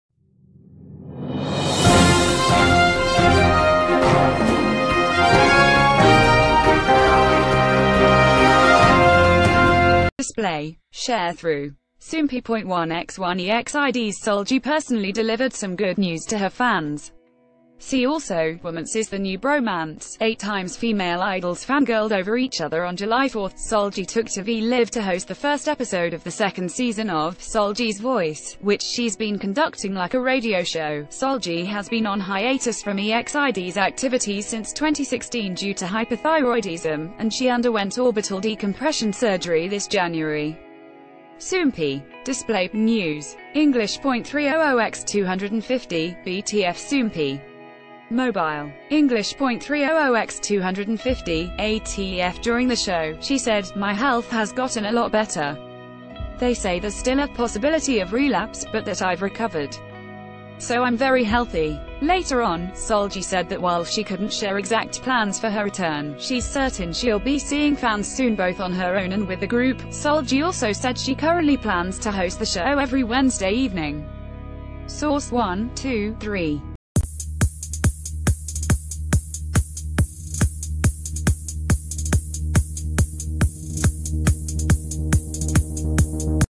EXID's Solji Gives Update On Her Health And Plans
Display. Sharethrough. Soompi.1x1 EXID’s Solji personally delivered some good news to her fans! See ...